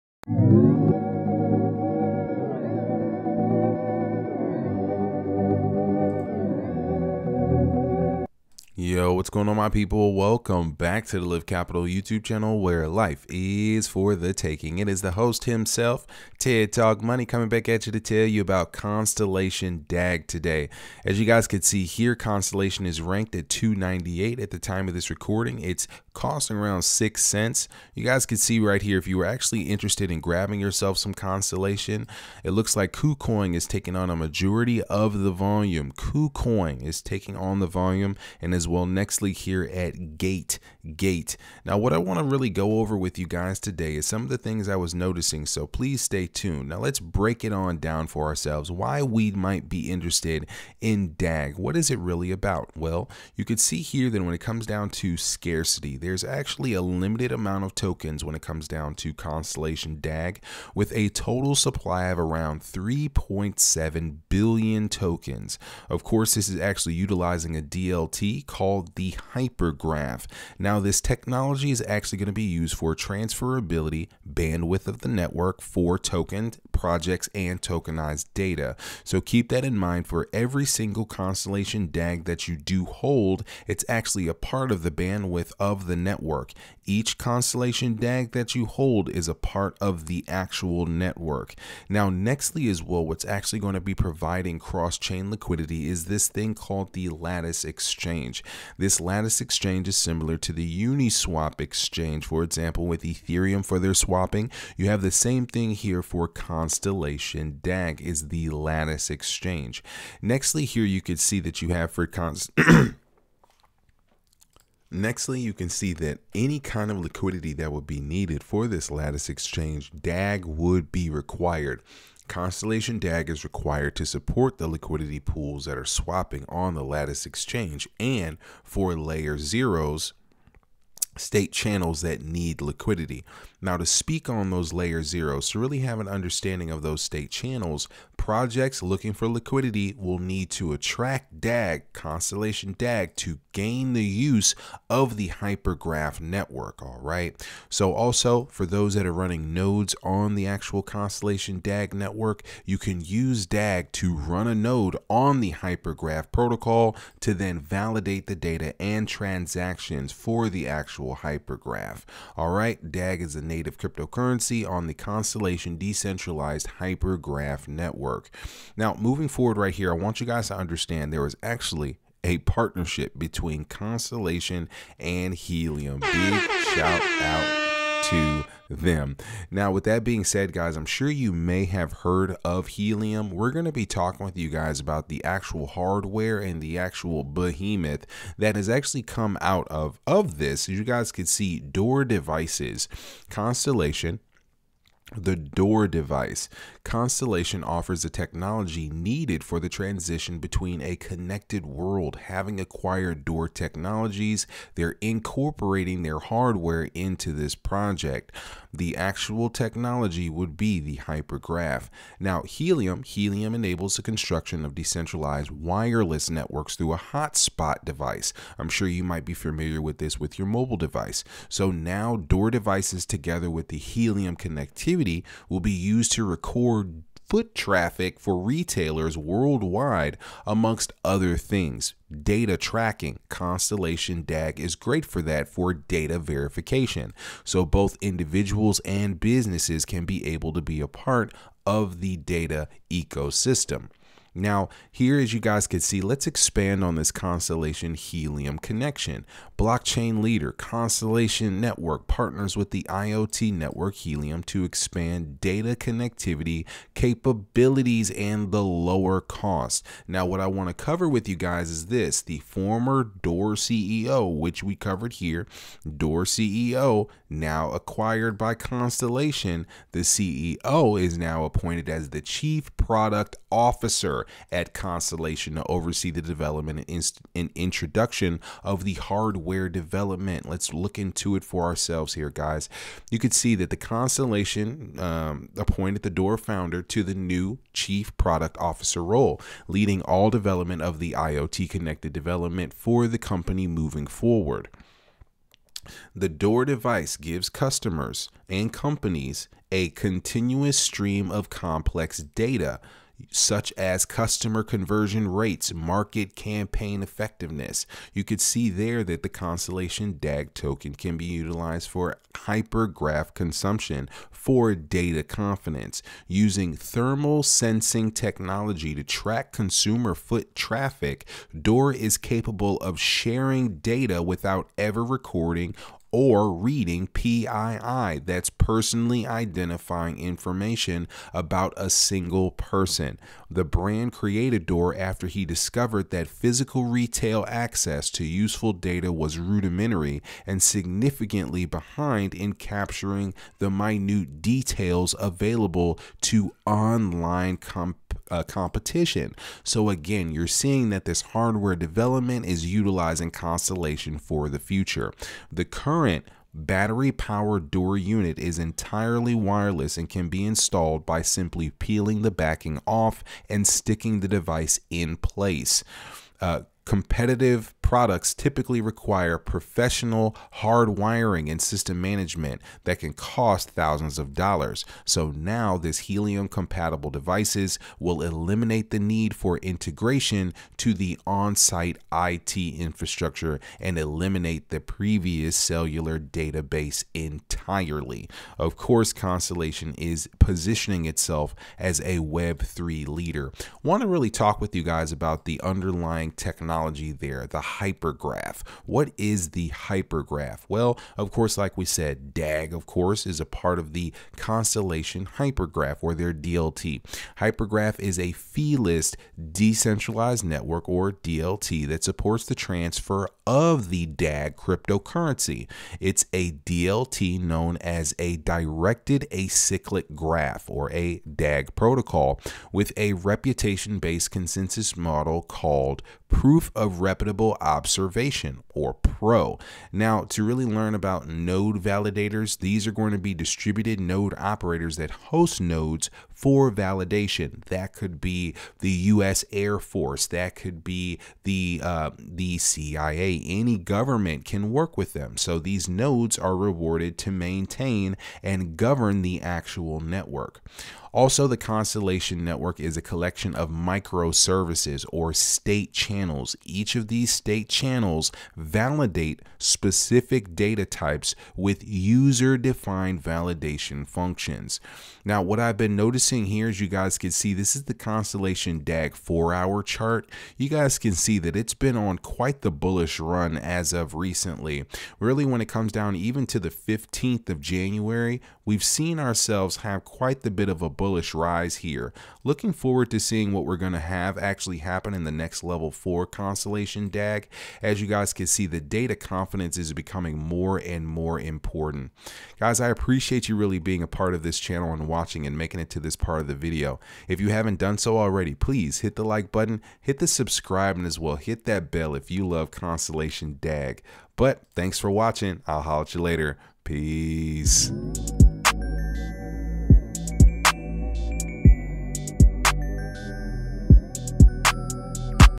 ⚙️ CONSTELLATION DAG ⚙️ A DEEP DIVE INTO THE DAG BASED HYPERGRAPH 🇺🇸
In this video, we take a look at Constellation DAG, a new crypto technology utilized by the US government for data tracking. This cutting-edge system utilizes a directed acyclic graph (DAG) to track and verify data transactions, making it highly secure and efficient. We discuss the features of this innovative new technology and its potential applications in various industries. Whether you're a crypto enthusiast or just interested in the latest technological advancements, this video is a must-watch.

LIFE IS FOR THE TAKING 🚀

We do not have a Whatsapp or Telegram